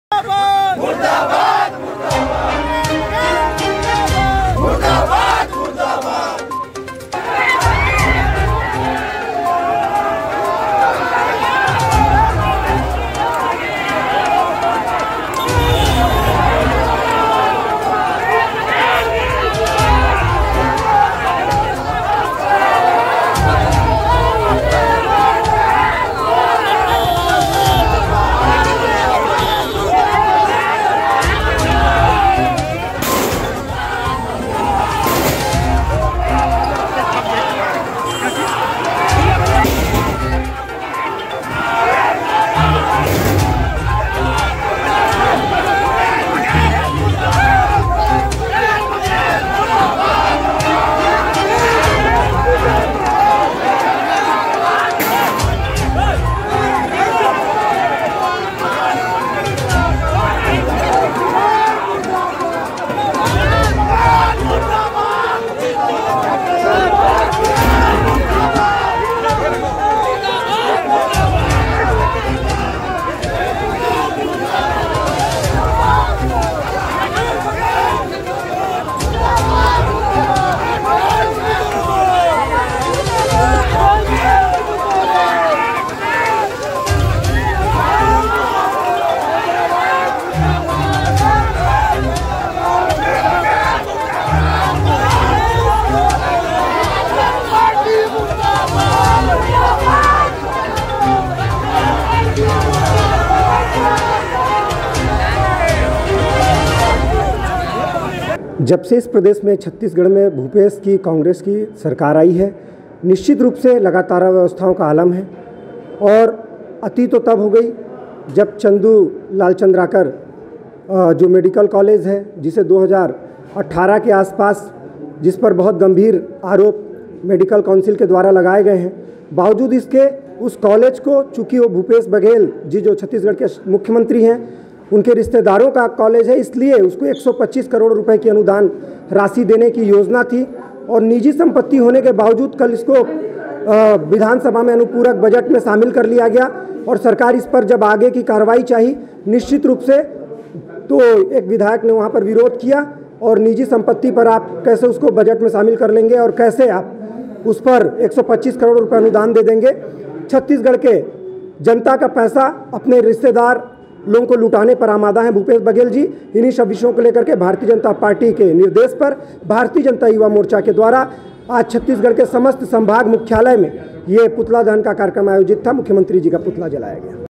भाजपा युवा मोर्चा ने किया भूपेश बघेल का पुतला दहन, कहा-कांग्रेस सरकार भाई-भतीजा वाद में लिप्त
अंबिकापुर। अम्बिकापुर के घड़ी चौक में भाजपा युवा मोर्चा ने भूपेश बघेल का पुतला दहन किया। इस दौरान पुलिस और भाजयुमो के कार्यकर्त्ताओं में झड़प भी हुई।। दरसअल प्रदेश भाजयुमो के आह्वान पर सभी संभाग मुख्यालय में भूपेश बघेल का पुतला दहन किया गया।